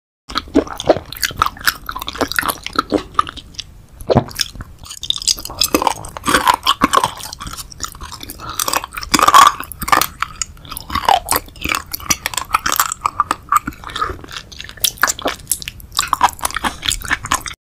ASMR RAW SEAFOOD
ASMR RAW SEAFOOD | RAW SQUID, SEA PINEAPPLE, SEA CUCUMBER | Eating Sounds Mukbang